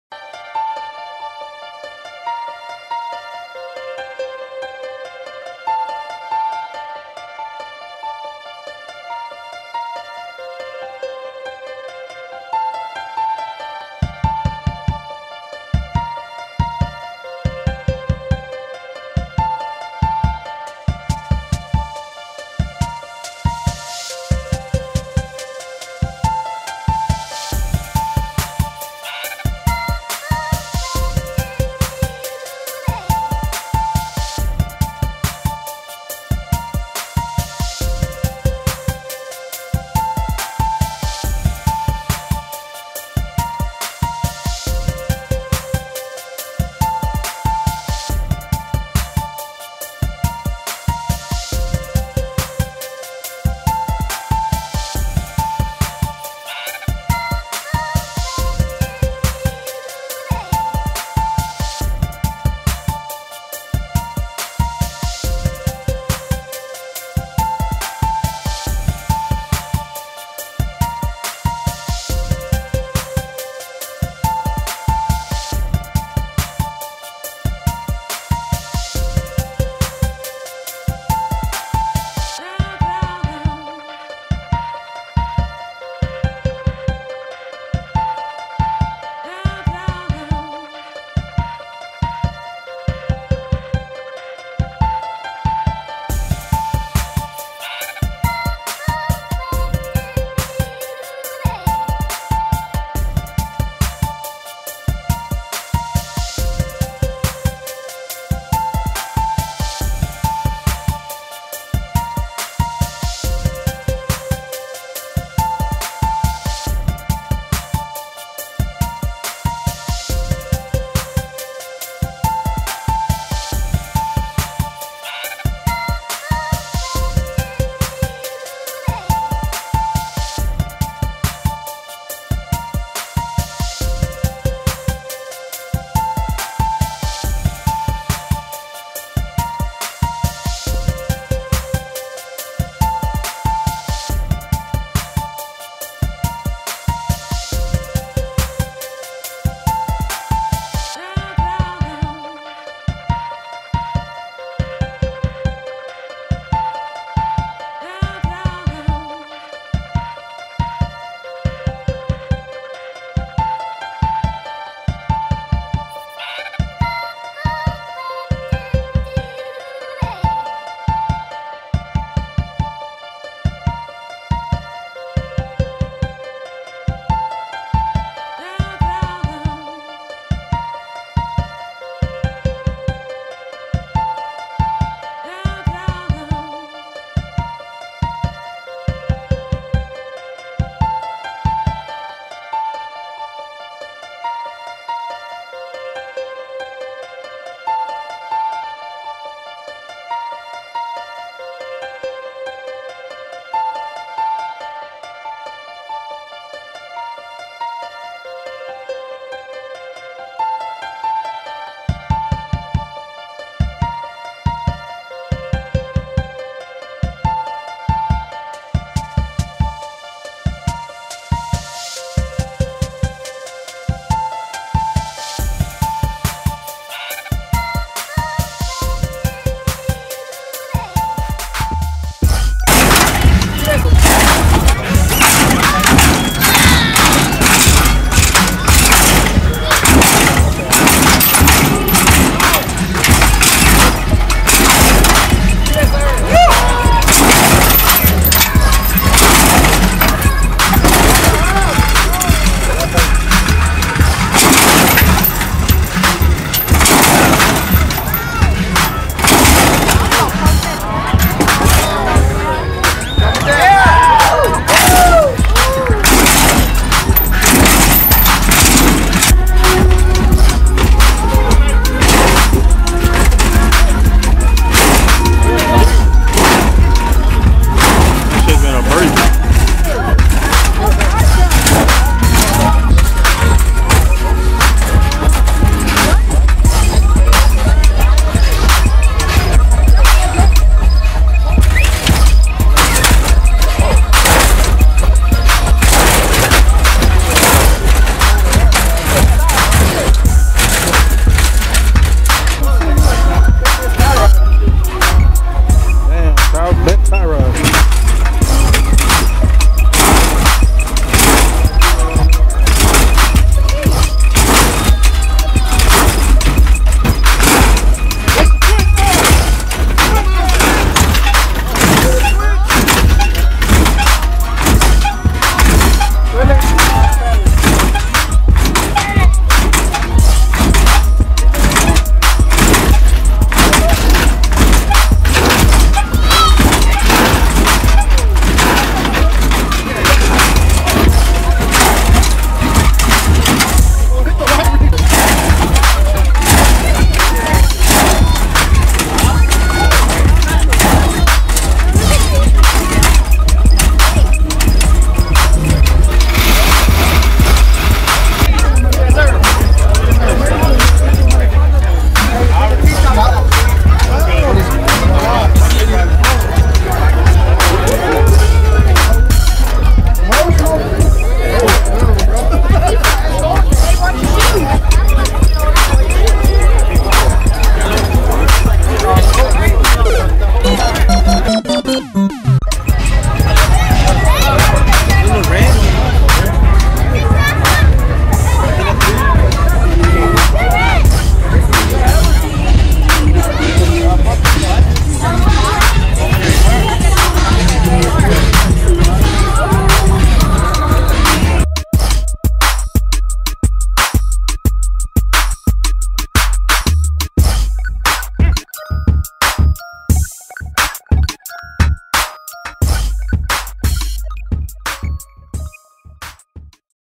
PAPAS BBQ 3/29/14 HOSTED BY THE MAJESTIX C.C.
lowriders cruising and having a good time famaliy oreinted at a local BBQ joint in Dallas TX